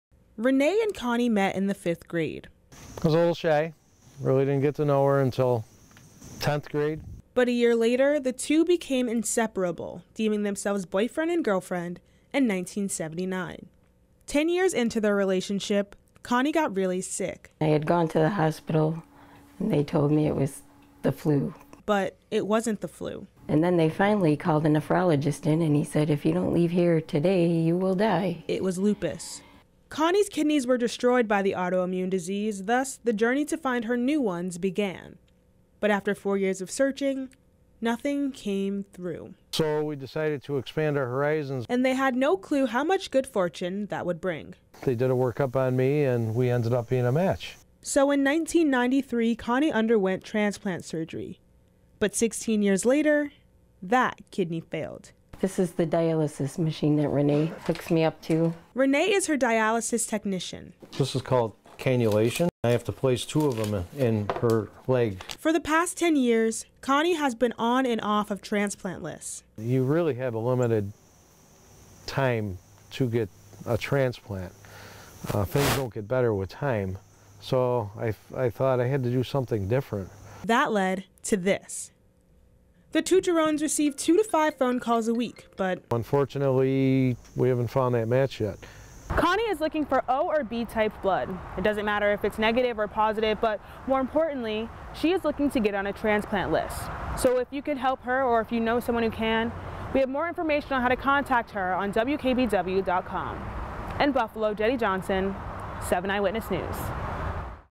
Husband goes through great lengths to find his wife a kidney
For the last 10 years, Kani has been on and off of transplant lists, so Rene decided he needed to do something radical and different to get Kani the kidney she needs.